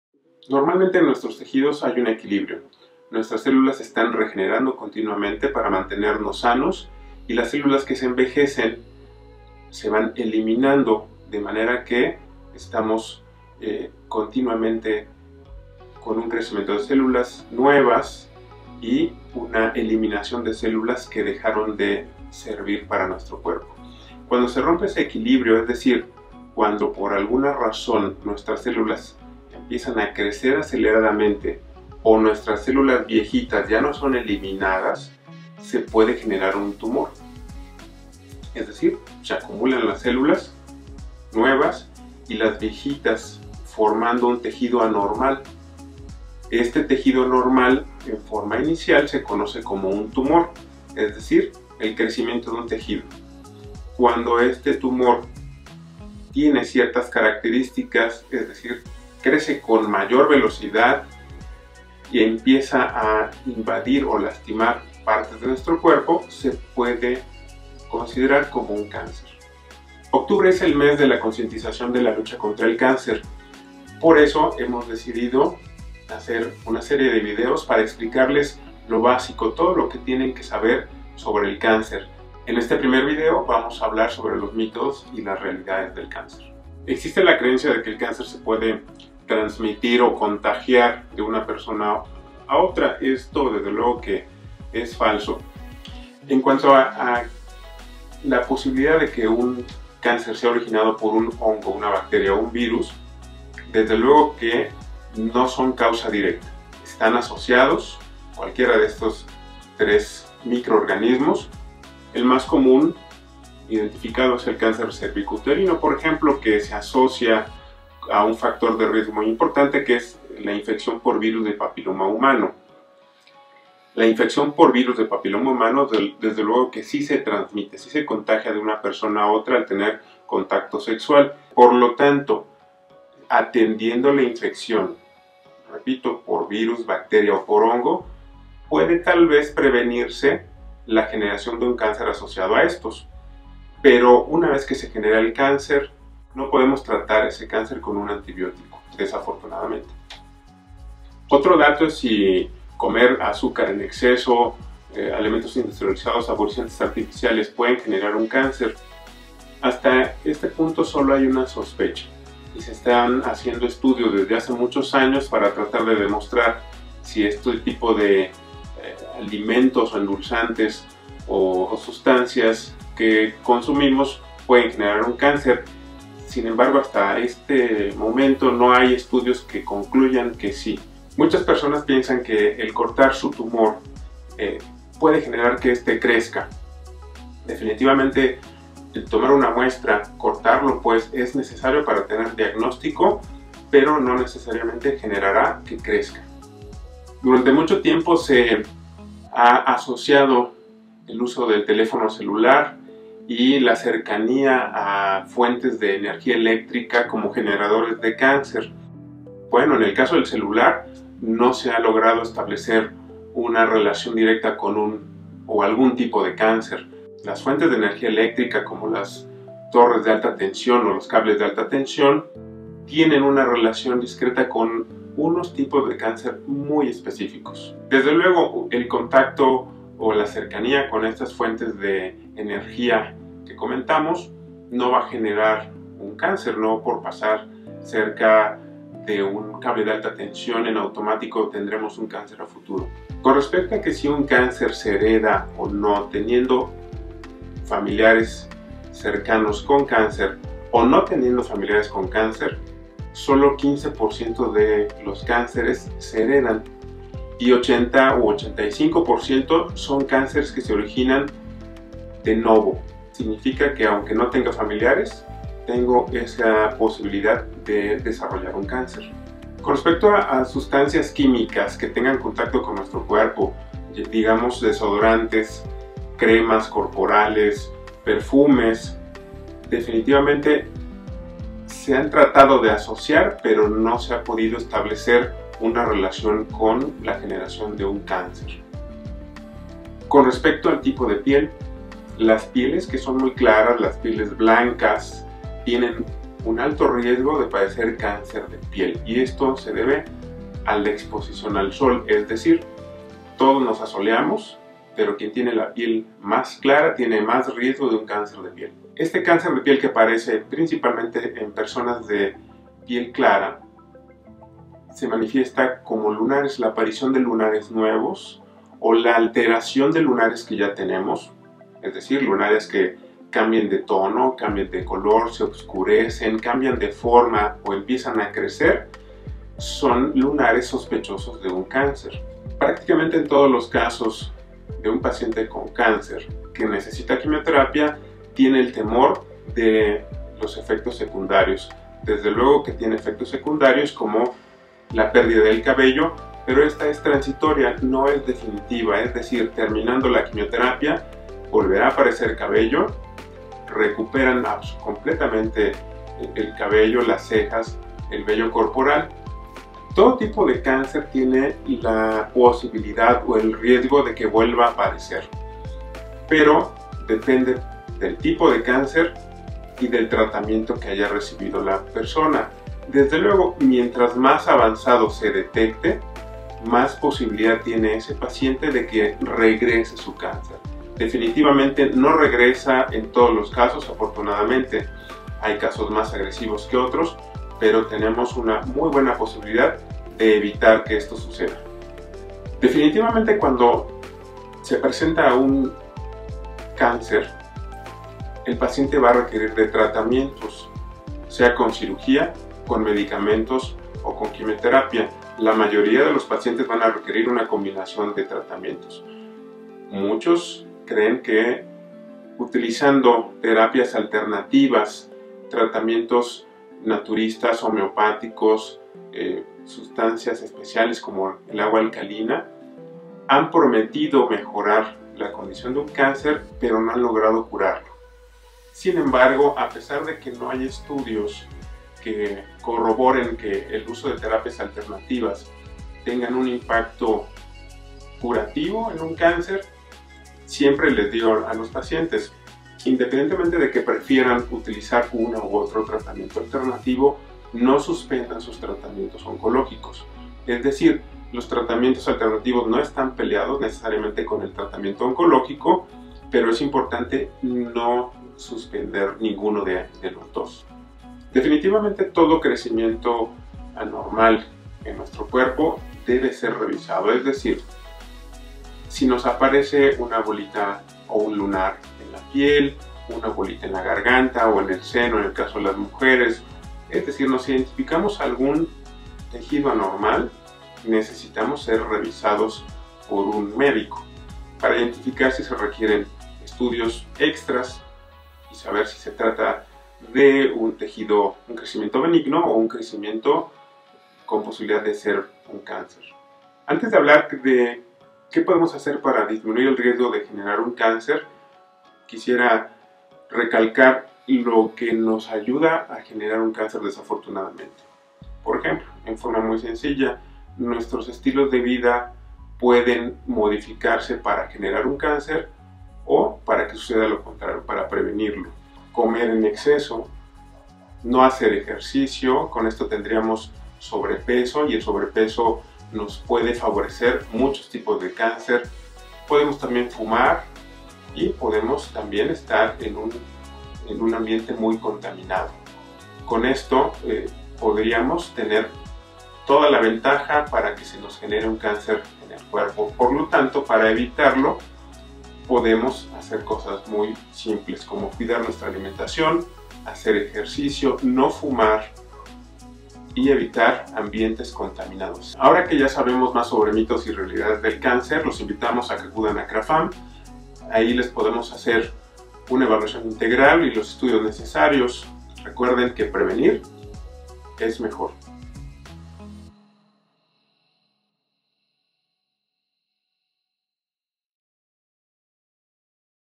El cáncer: Mitos y realidades
Octubre es el mes de la concientización sobre el cáncer, por eso en Creafam hemos creado esta serie de videos con todo lo que debes saber sobre el cáncer.

En este primer video el Dr. Carlos Cortés, cirujano oncólogo nos explica los mitos y realidades del cáncer.

Mira todos los videos de la serie "Todo sobre el cáncer"

Creafam 💜 Los expertos en SueñosCumplidos

Fe de erratas: En el video el Dr. Carlos Cortés aparece como "Cirujano Oncológico, Ginecología y Obstetricia" su título debería ser "Cirujano Oncólogo, Ginecología Oncológica"

Dr. Carlos A. Cortés García
Cédula profesional: 3011774
Cédula especialidad: 5179353
Cédula subespecialidad: 5554101
• Subespecialidad en Cirugía Oncológica.
  Centro Médico Nacional “Siglo XXI”.
• Especialidad en Cirugía General.
  Benemérita Universidad Autónoma de Puebla.
• Médico Cirujano y Partero.